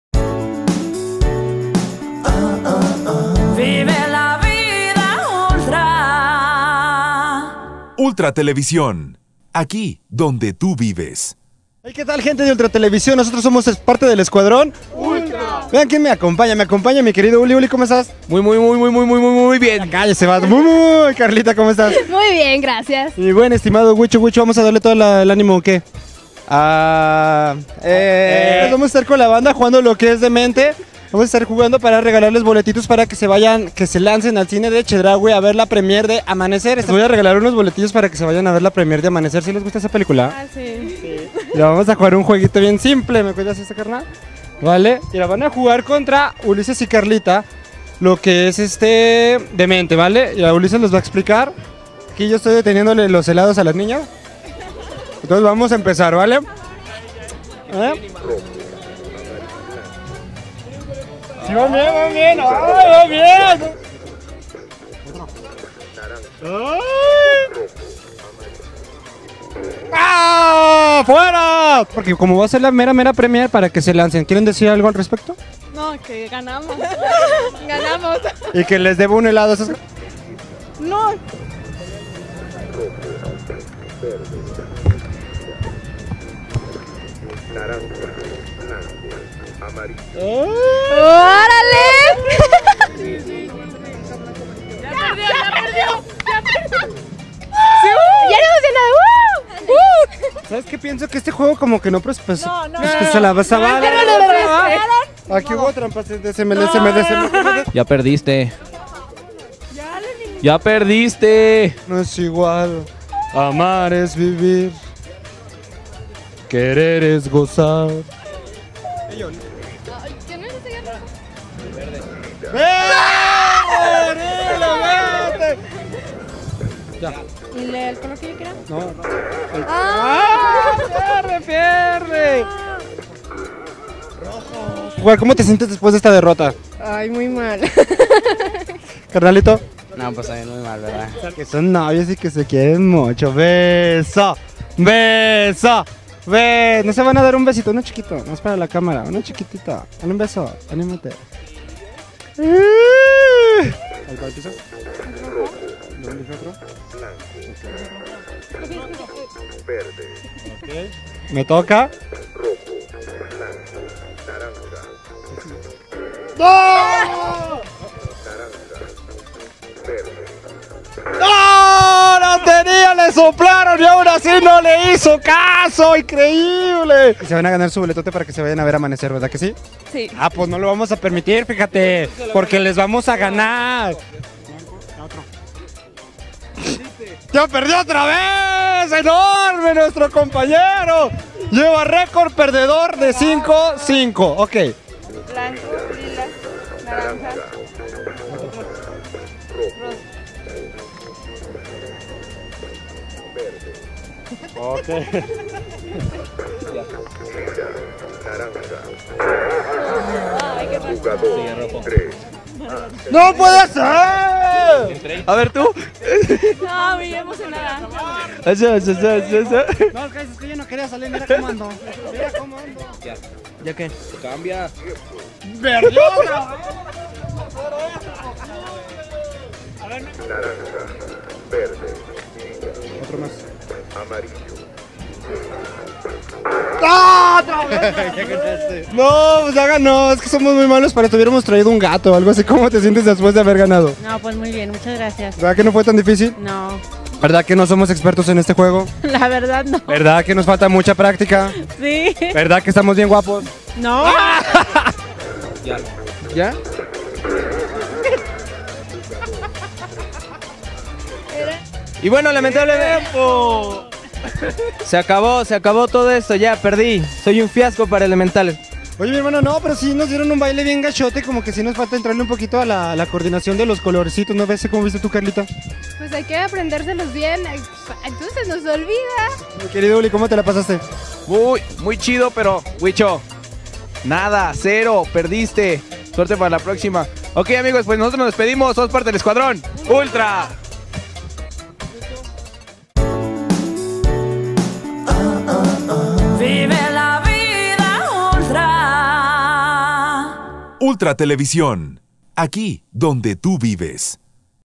Boletos para Amanecer
Los chicos del Escuadr&oacute;n se fueron a las calles de la ciudad de Aguascalientes a jugar demente, y a los que lograron vencerlos les regalaron sus boletos para la pel&iacute;cula de Amanecer cortes&iacute;a de Ultra Televisi&oacute;n.